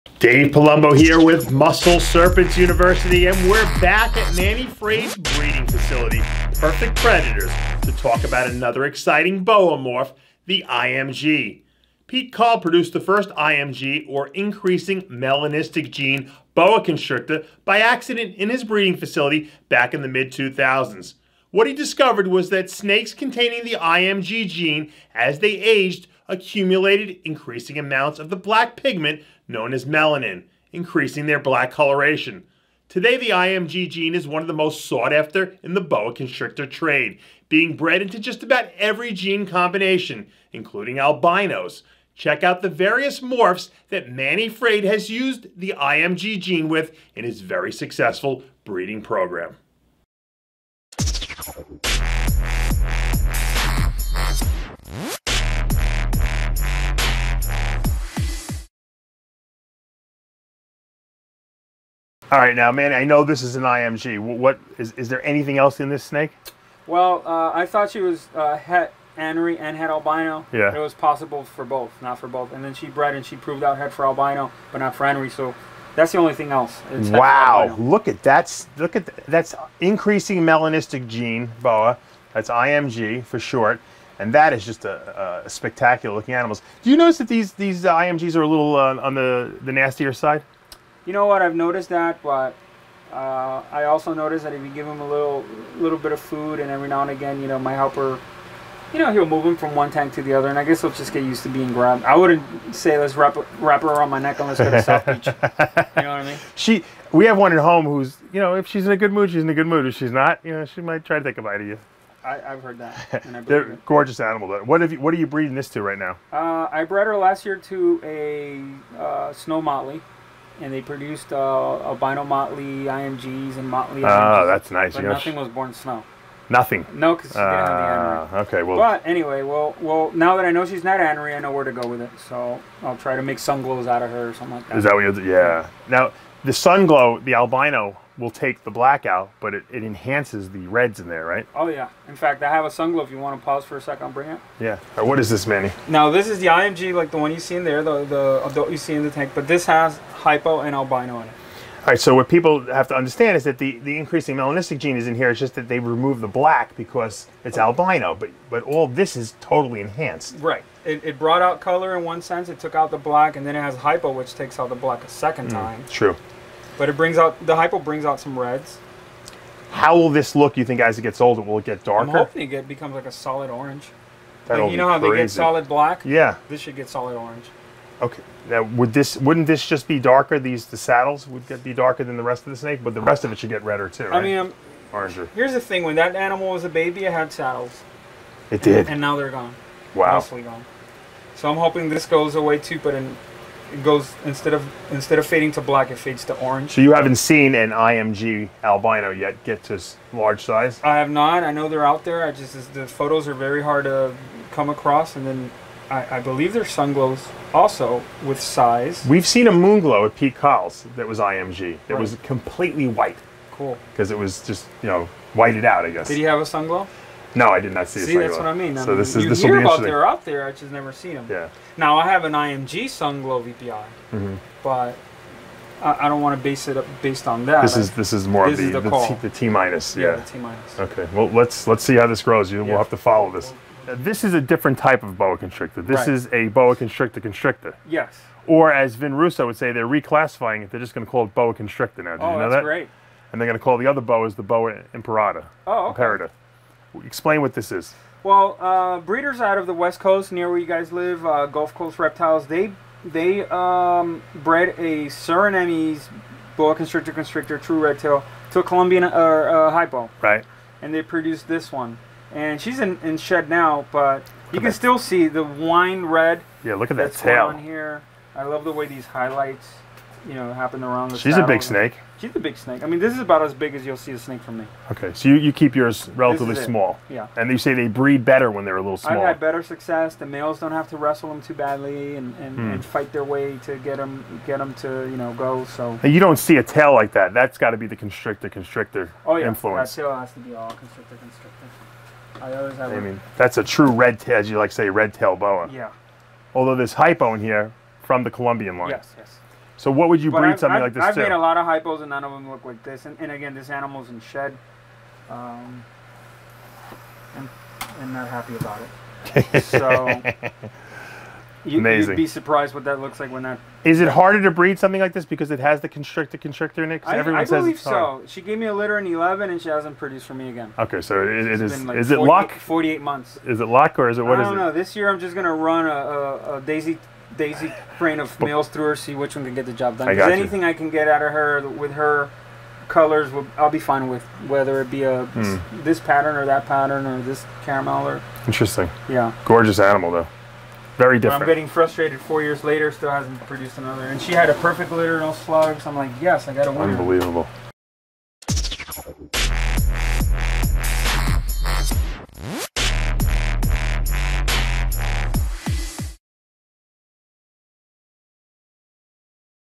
Black Boa Constrictors! IMG Gene Education at Perfect Predators- Part 4
Dave Palumbo of Muscle Serpents University investigates the IMG (increasing melanistic gene) Boa Constrictor morph at Perfect Predators.  Also, get a look at the Hypo Suriname Boa Constrictor Constrictor!